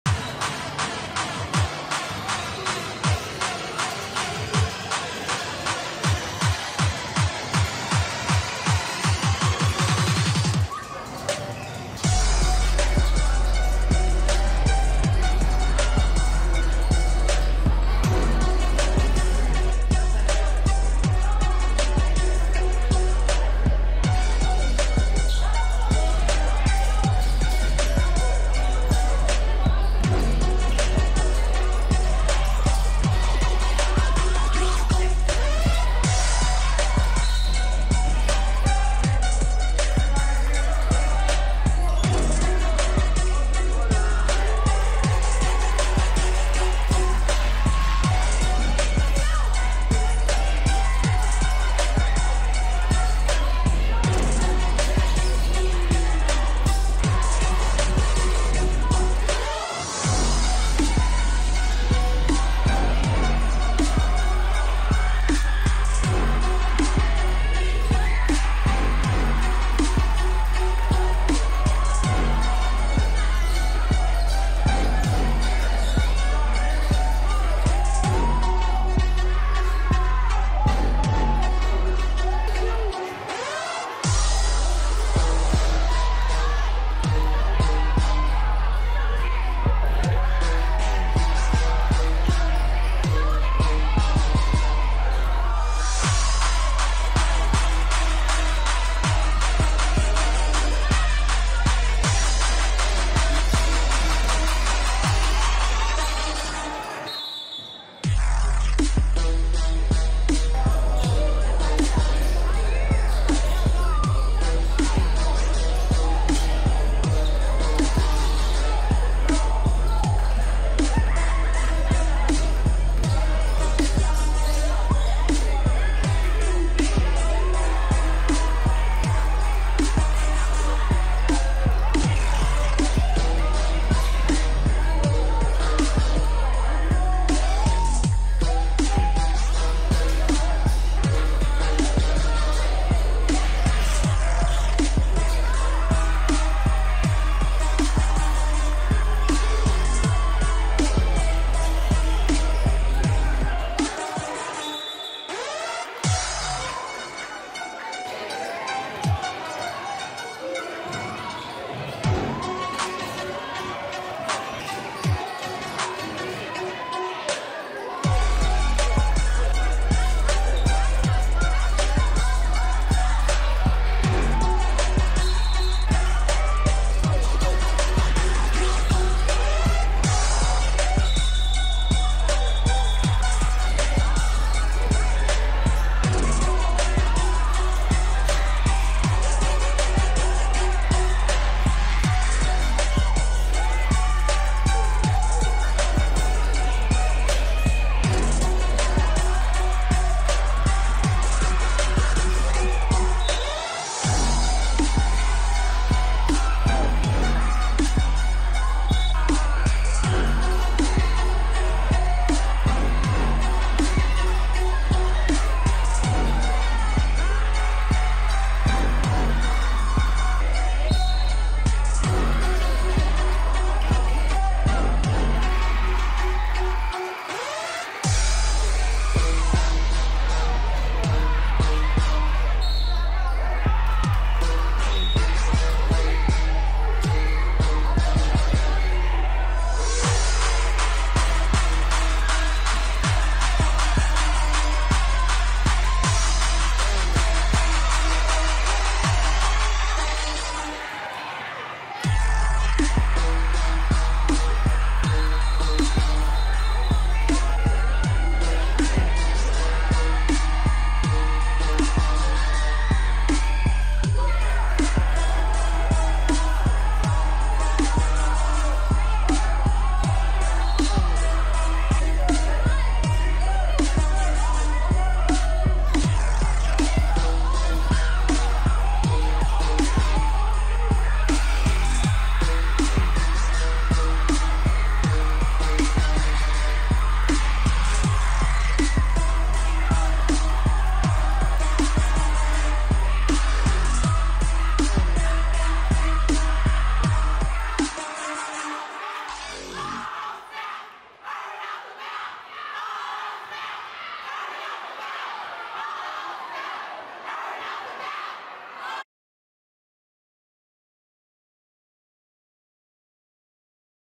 HueyTown Panthers vs Erwin bball Highlights
This Middle School Matchup Went down to the wire. Hueytown panther fight to come from behind late in second half to pull out a close victory. @YungPlayaEntertainment